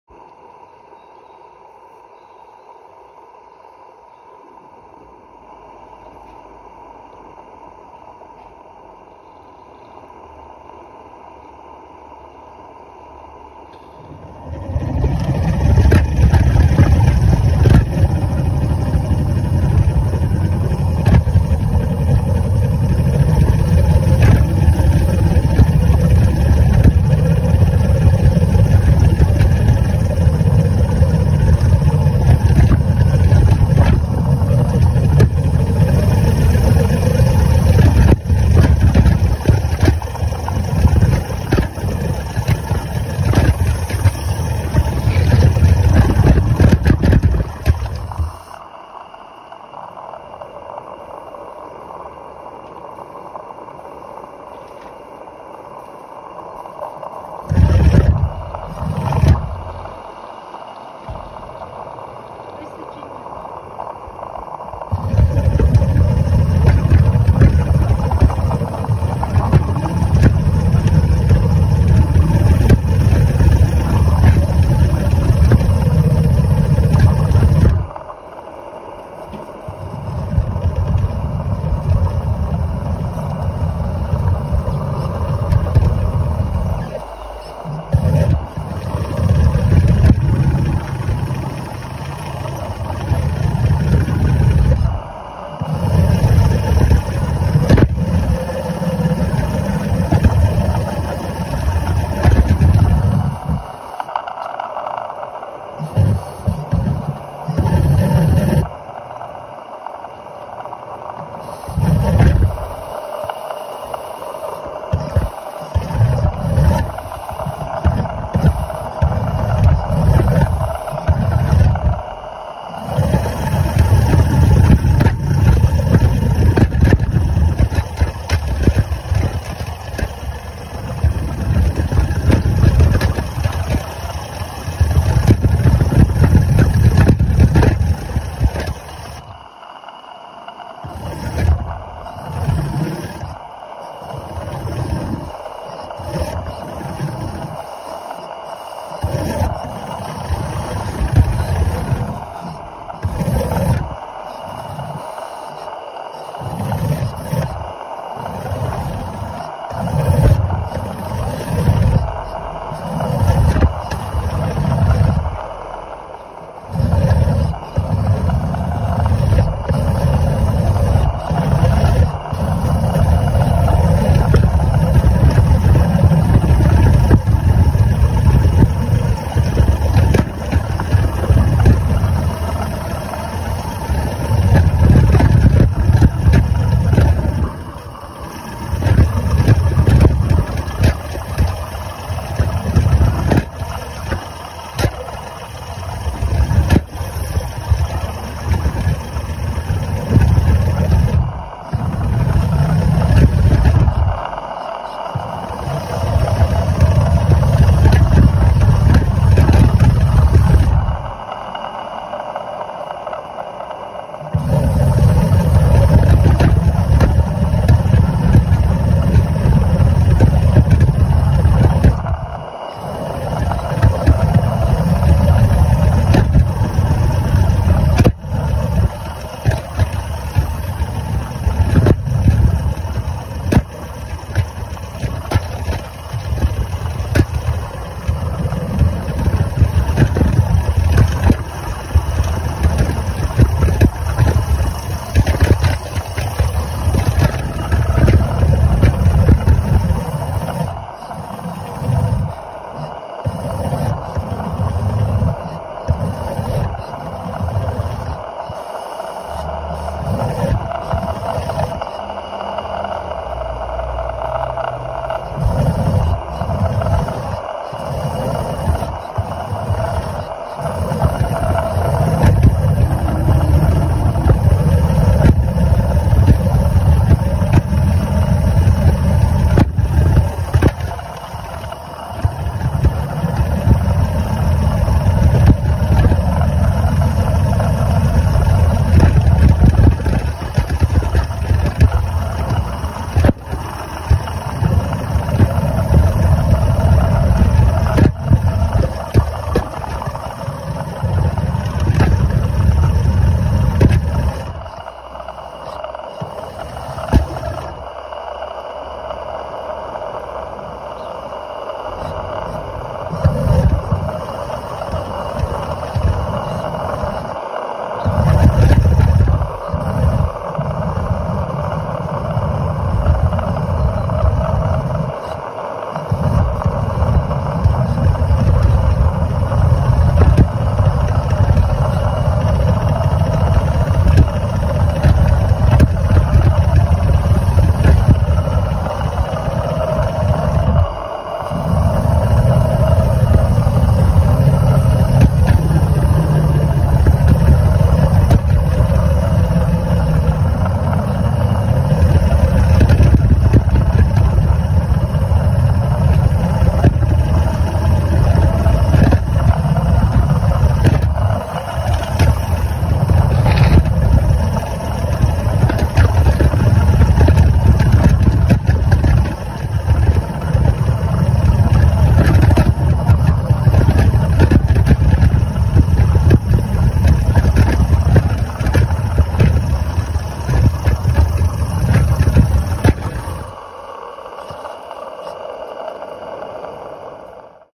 Wagon ride around the railway
Radio controlled Maxitrak 3/4” Sentinel DG6 steam wagon trundles around the FeckHallBahn’s road accessible areas, dropping off and picking up loads from wine to wool and most things in between. Sue Gray’s dog - Bach - has hopped along for the ride.